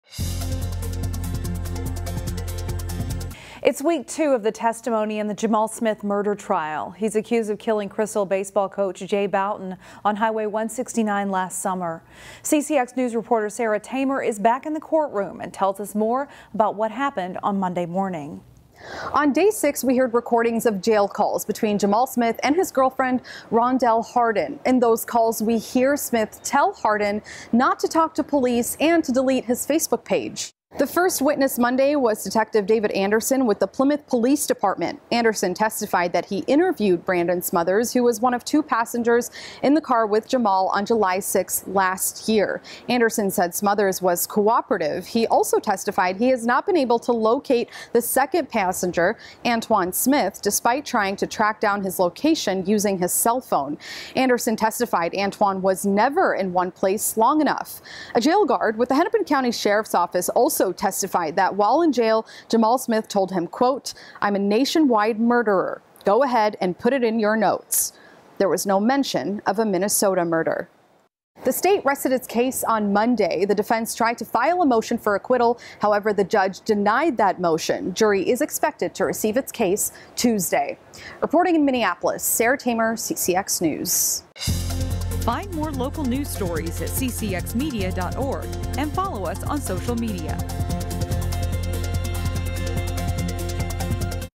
Day 6 of Jamal Smith Murder Trial: Smith Testifies, Denies Shooting and Killing Jay Boughton
Jamal Smith took the stand in his own defense, saying he wasn’t the one who shot and killed Jay Boughton on Hwy. 169 last summer. Plymouth Police Detective David Anderson also testified regarding the other passengers and calls Smith made from jail. The case is expected to go to the jury on Tuesday.